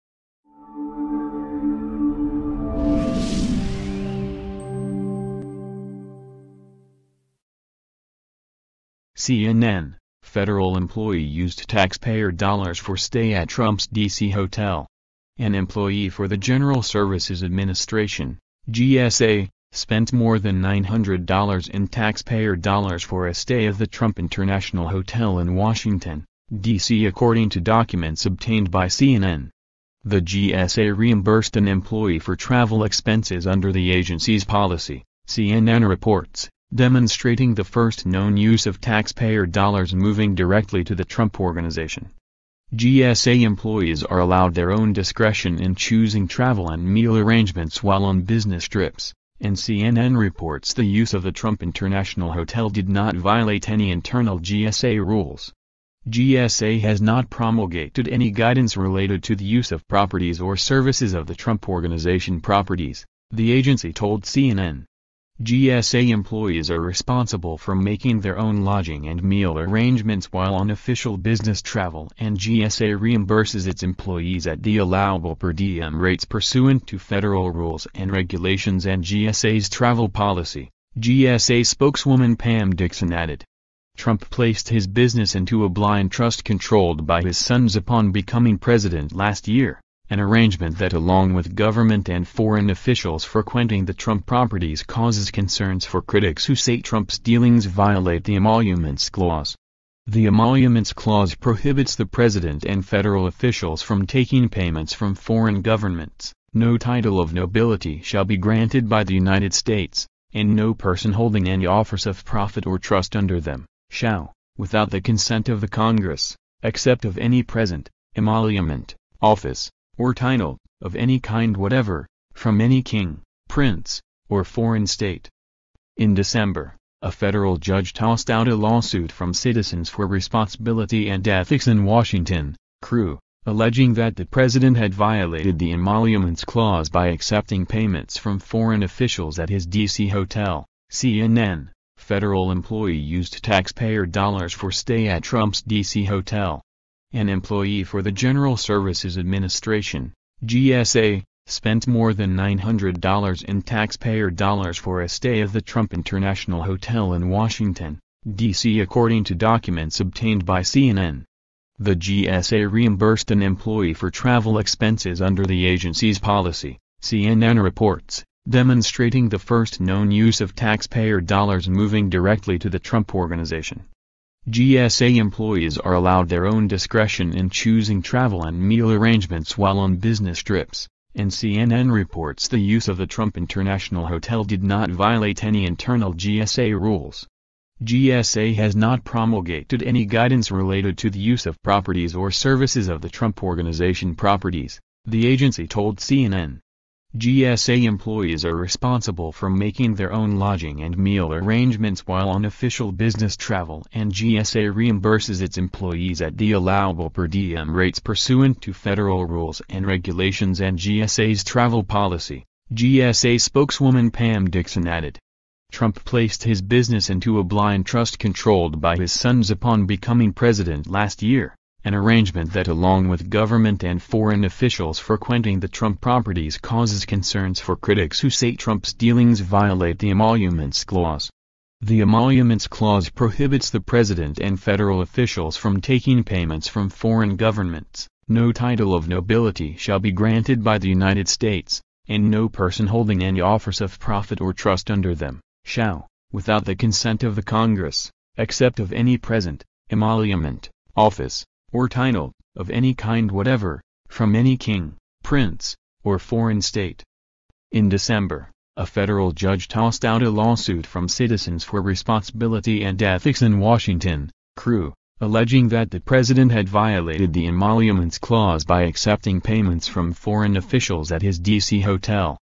CNN Federal employee used taxpayer dollars for stay at Trump’s DC hotel
CNN: Federal employee used taxpayer dollars for stay at
Trump’s DC hotel. An employee for the General
Services Administration (GSA) spent more than $900 in
taxpayer dollars for a stay a the Trump International
Hotel in Washington, D.C. according to documents
obtained by CNN. The GSA reimbursed an employee for
travel expenses under the agency's policy